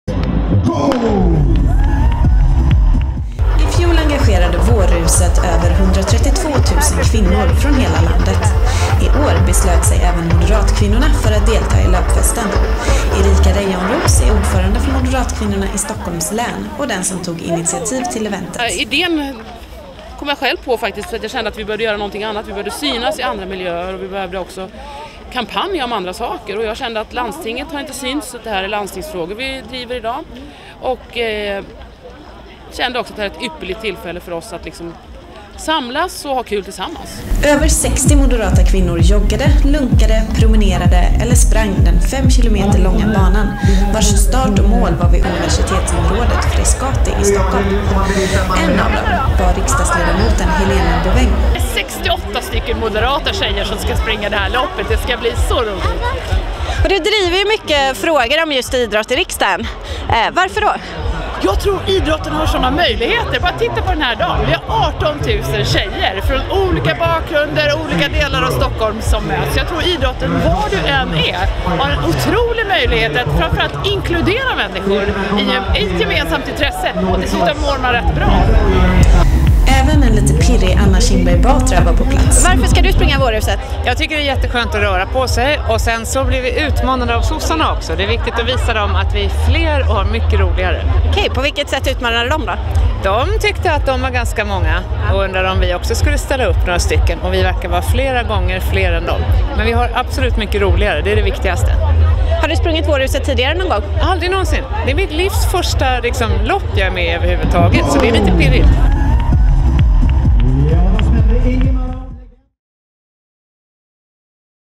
Moderatkvinnorna och vårruset 2012
Över 18000 kvinnor sprang Vårruset i Stockholm 2012. 68 av dem är Moderater och engagerade i Moderatkvinnorna. Vi passade på att ta pulsen på några av dem alldeles innan startskottet.